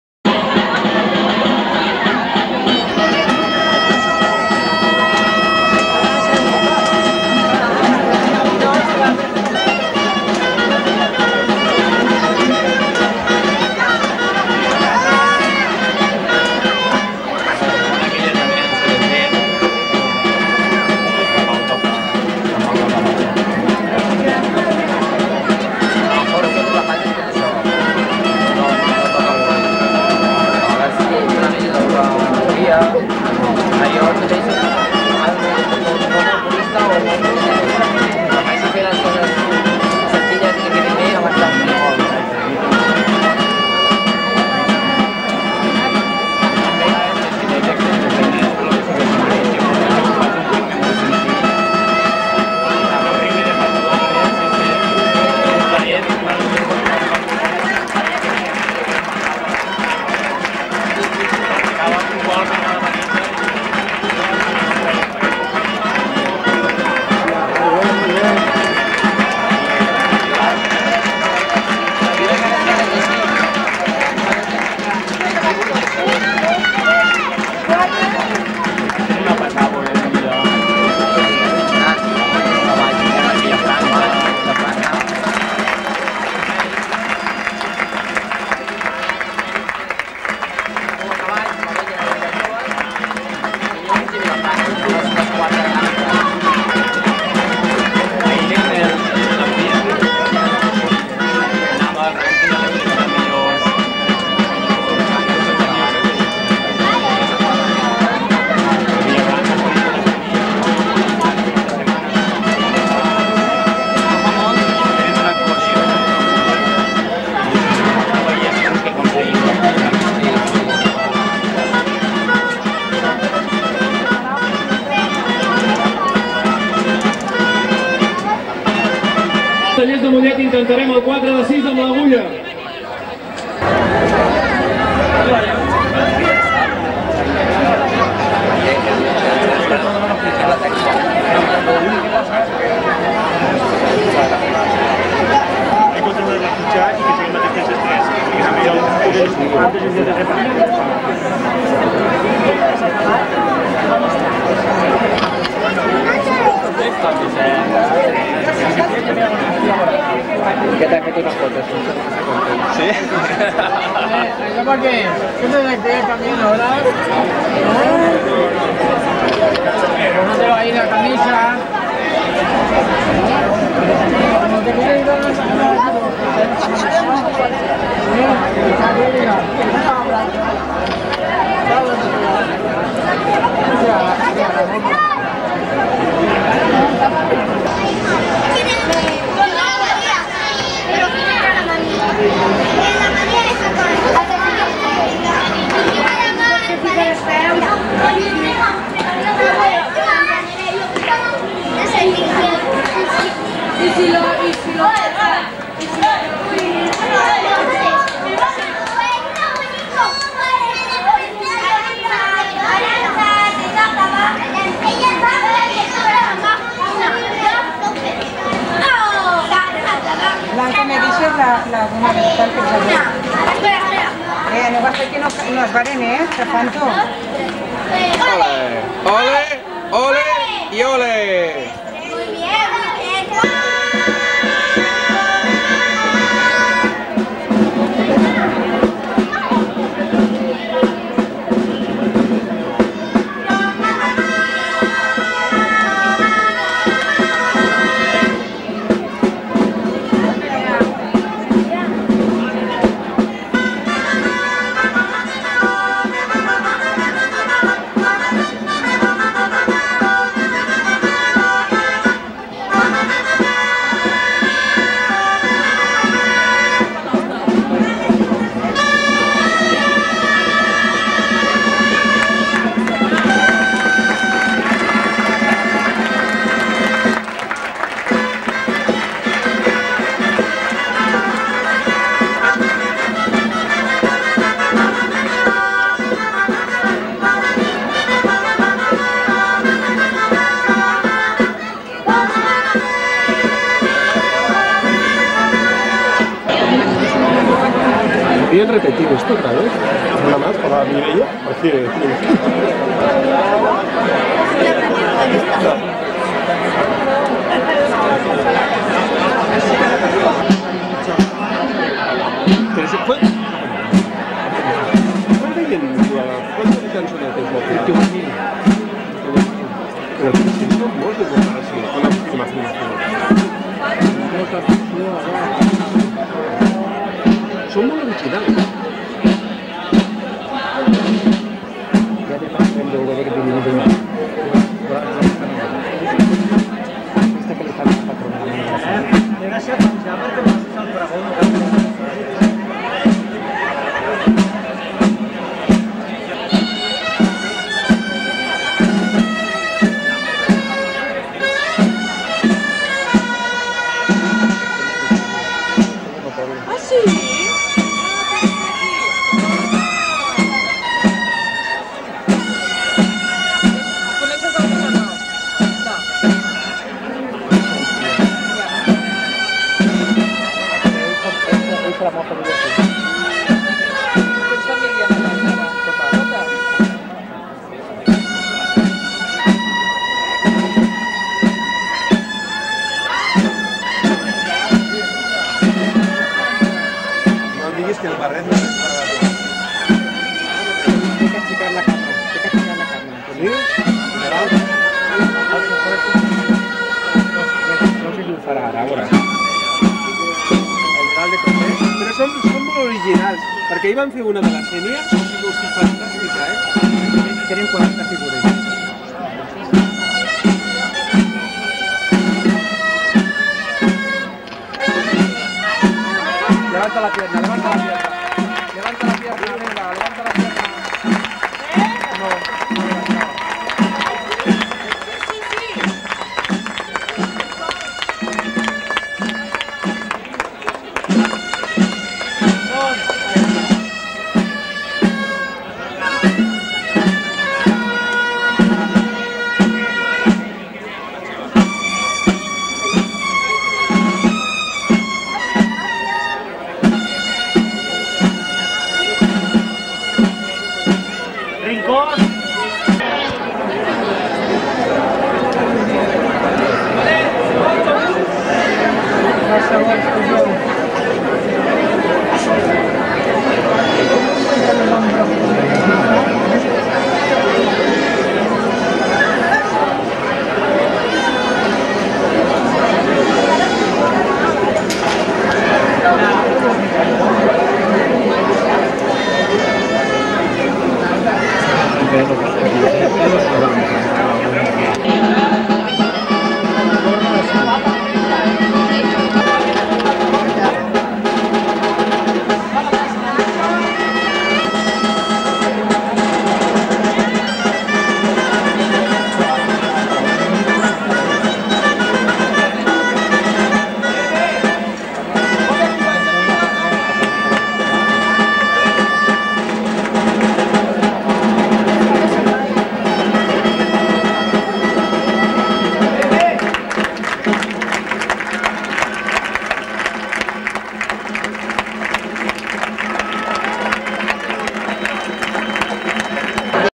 Diada Castellers de Mollet (2/3)
Un molletà-algemesinenc, Jose Poblador, va gravar aquestes imatges de la nostra estada a Mollet.